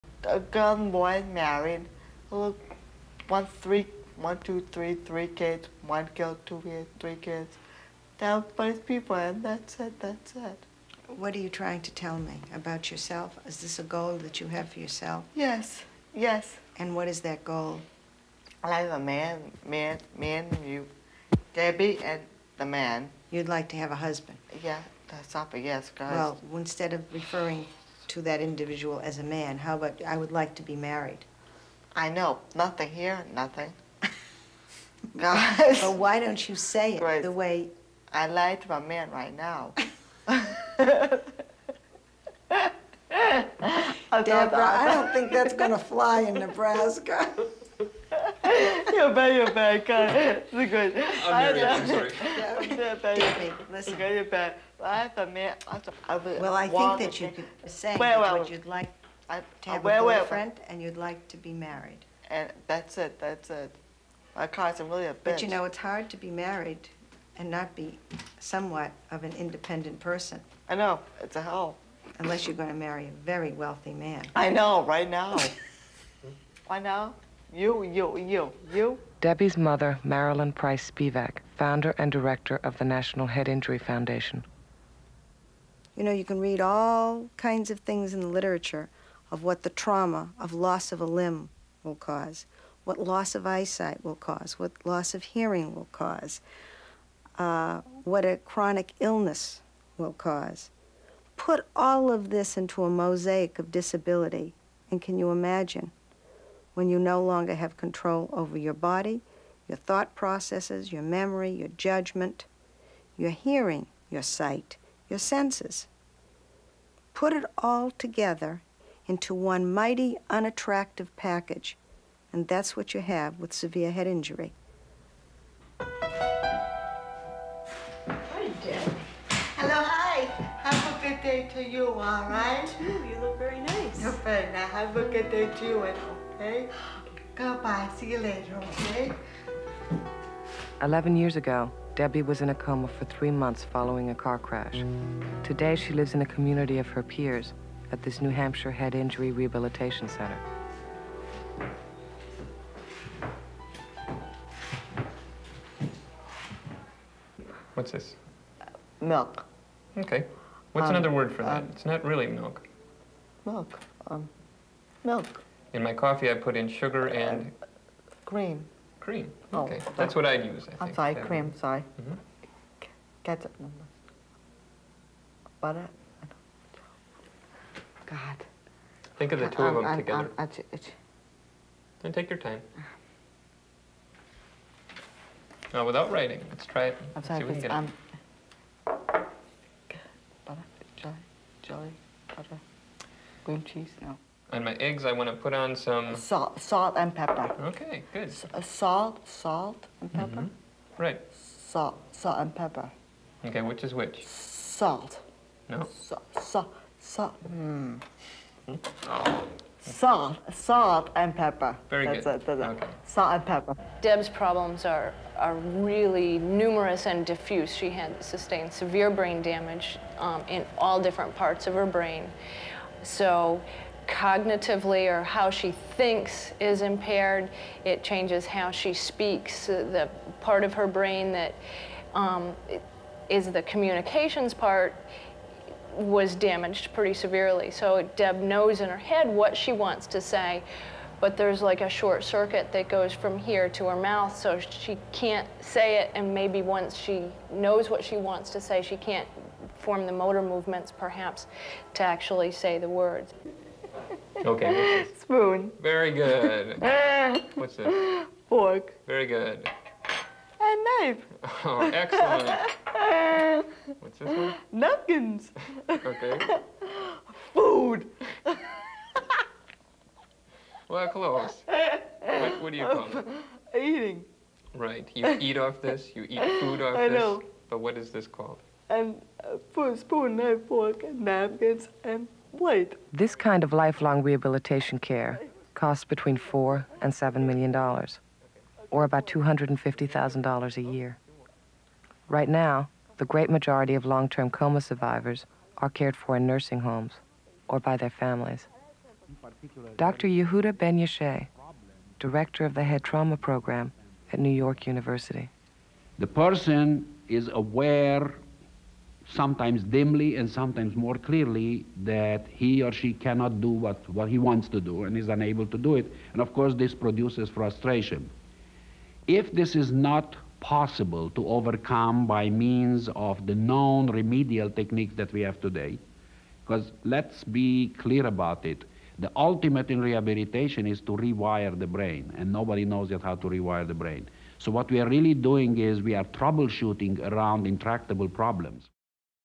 Trauma Teen Warns - Don't Drink or Text & Drive
to get the entire 1 hour film. To support my efforts to create more clips please donate to me Teenagers who have had a severe head injury struggle to return to some form of normal life. This film tells the story from the high-level experts to ordinary citizens who live with this. It was a PBS primetime television special.